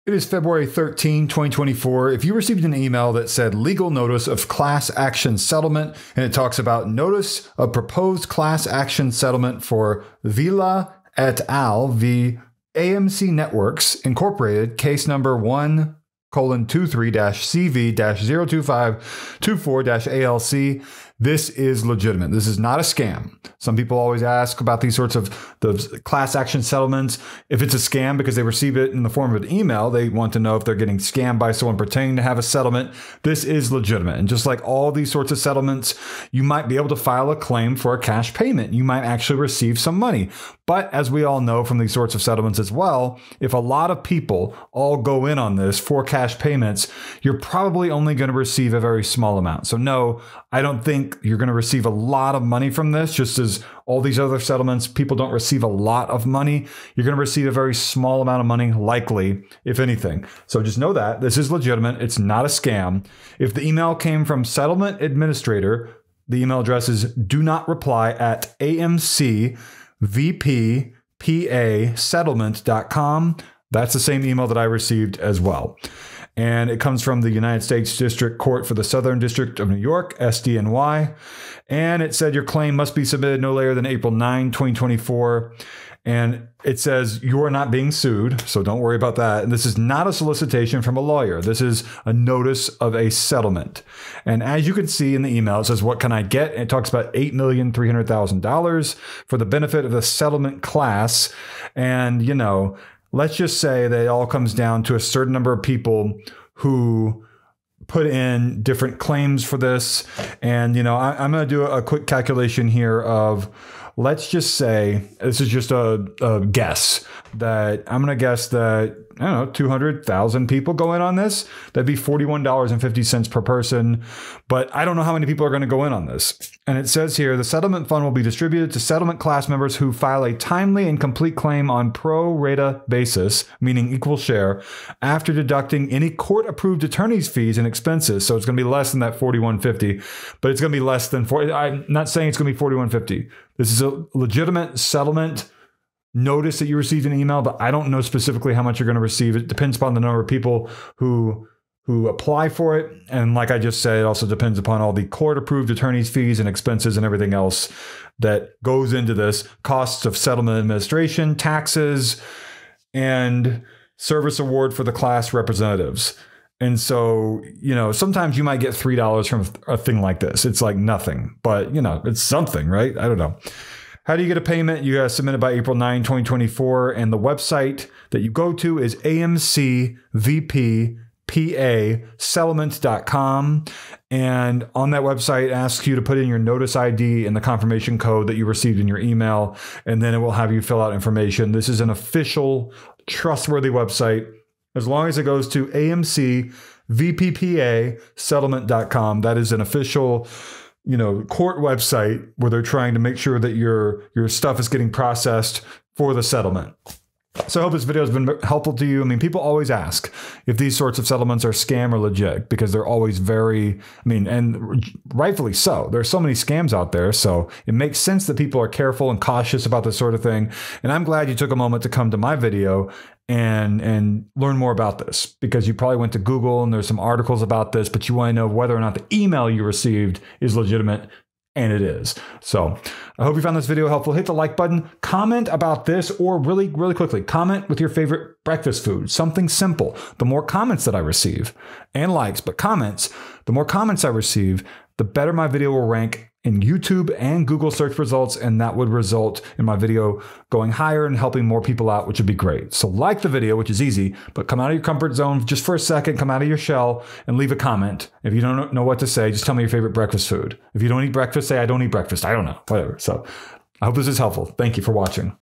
AMC Class Action Lawsuit Settlement: Scam or Legit? (2024)
You're probably wondering if the AMC Networks class action lawsuit settlement email from a settlement administrator that leads to amcvppasettlement.com is a scam or legit. In fact, it's legit. The email's title is, "Legal Notice of Class Action Settlement." It comes from "Settlement Administrator." It then reads as follows:

"NOTICE OF PROPOSED CLASS ACTION SETTLEMENT. Vela, et al. v. AMC Networks, Inc., Case No. 1:23-cv-02524-ALC
(United States District Court for the Southern District of New York)

Our Records Indicate You May Be Entitled to a Payment from a Class Action Settlement Because You Were a Registered User of One of the Following “AMC Services”:  (1) AMC+, (2) Shudder, (3) Acorn TV, (4) ALLBLK, (5) SundanceNow, or (6) HIDIVE

Click HERE To File A Claim for a Cash Payment"

The email then continues:

"This notice is to inform you that a settlement has been reached in a class action lawsuit claiming that Defendant, AMC Networks, Inc. ('AMC'), disclosed the personally identifiable information of registered users of AMC Services (1) AMC+, (2) Shudder, (3) Acorn TV, (4)) ALLBLK, (5) SundanceNow, and (6) HIDIVE to Third-Party Tracking Companies, without their consent, in violation of the Video Privacy Protection Act (the 'VPPA'). Personally identifiable information includes information which identifies a person as having requested or obtained specific video materials or services from a video tape service provider. AMC denies that it violated any law. The court has not determined who is right, but the parties have agreed to the settlement to avoid the uncertainties and expenses associated with continuing the case."

It's likely you won't receive anything too much from the settlement but give it a shot anyway.